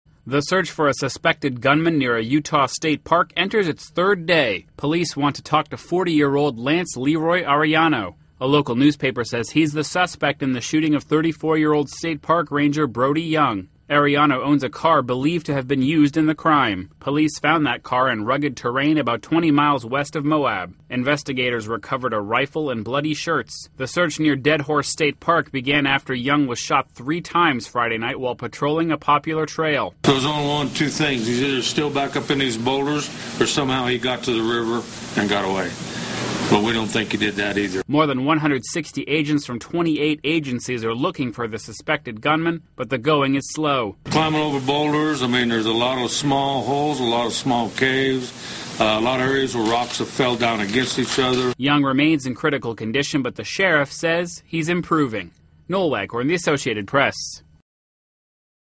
Utah Park Ranger Critically Hurt in Shooting
Police are spending a third day searching for a gunman suspected of critically wounding a Utah state park ranger. The search is going on in a rugged canyon along the Colorado River about 20 miles west of Moab. (Nov. 22)